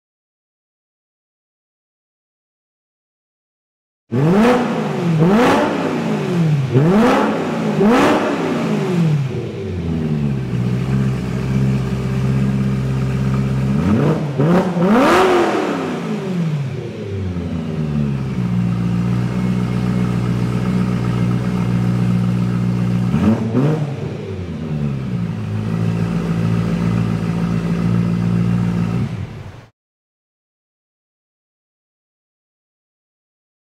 BMW E92 M3 No Mufflers
We are modifying this BMW's mufflers, so while they were off we decided to start it up and see what it sounded like. Enjoy.